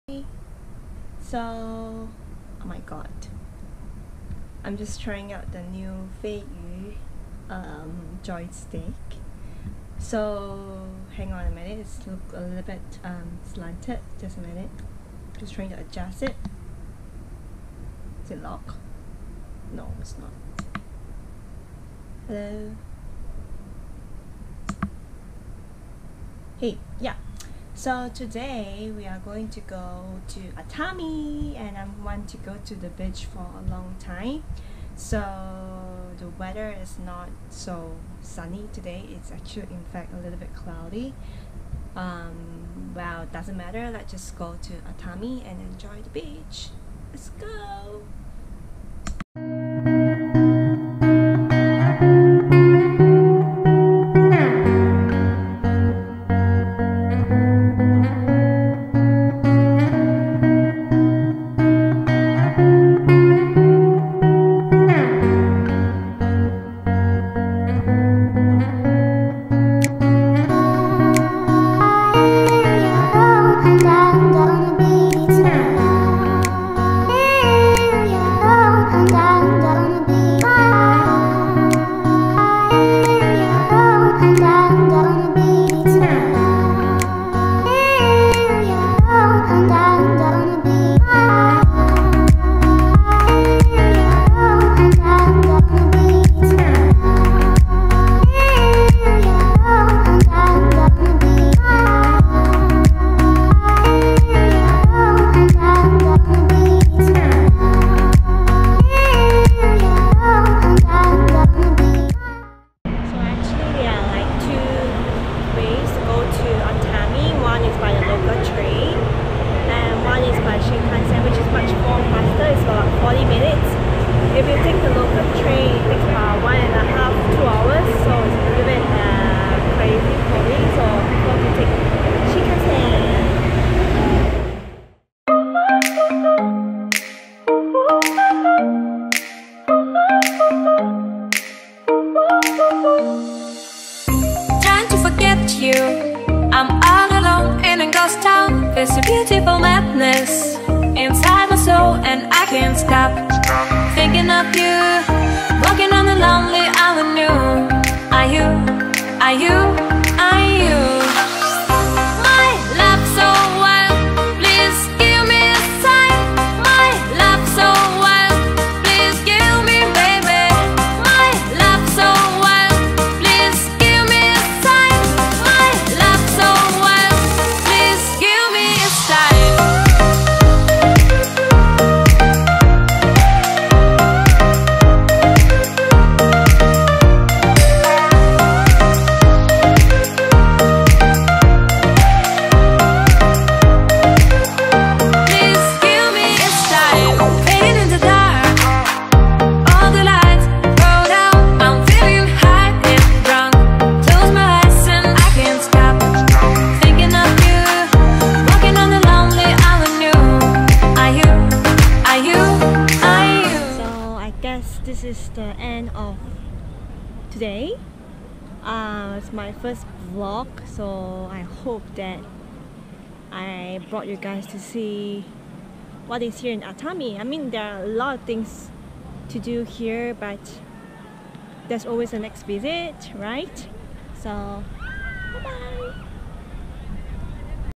Living Japan Vlog #1 - Let’s go to Atami !
This is my first vlog and I’m doing this to record down my moments in Japan! I hope to share my travel experiences with everyone ! Still an amateur in video editing, hope to get better at each video ;)

An attempt to the beach at Atami. Weather wasn’t being nice at me, have you seen people wearing raincoat to the beach ? Yup that’s right, I wore a raincoat. Questioned myself so many times “why am I doing this ?” . Sometimes we don’t know why, but we just feel like doing it. Have you feel like that before ?